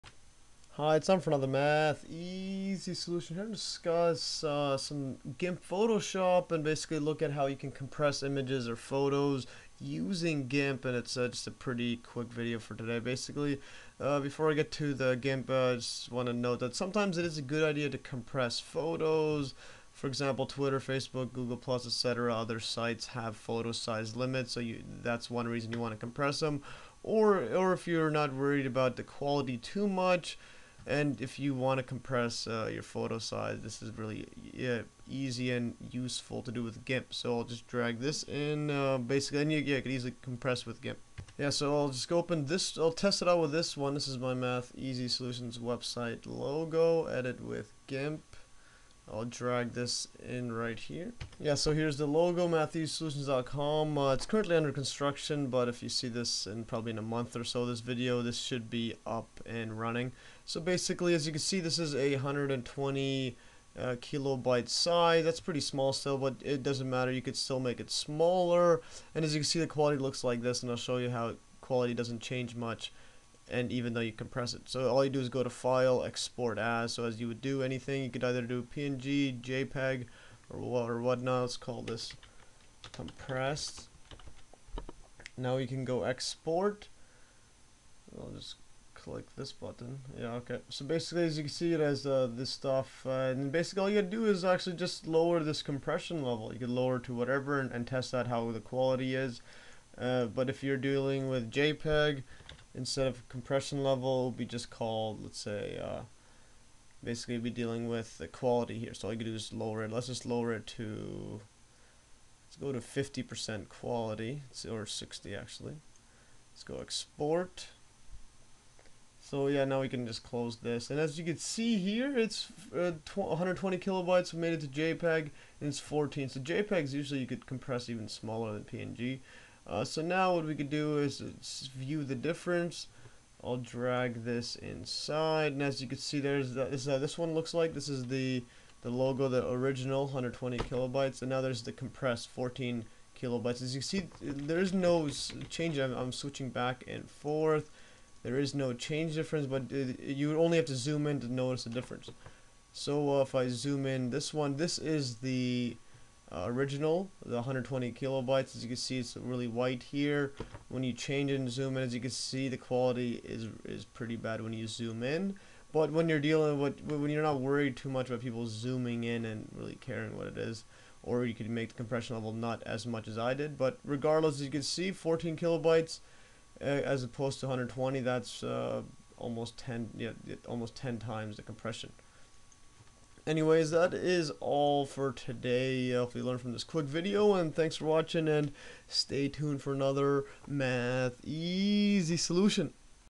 GIMP Photoshop: Compressing Images
In this video I go over the basics in compressing images with the powerful and free GIMP Photoshop editing software. Basically you can change the quality or compression level of any PNG, JPG, GIF, or other image formats simply from the Export As... dialog box. Compressing images is create for emails or uploading to social media sites where there are size limitations. It is also useful to simply save space on your computer hard drive!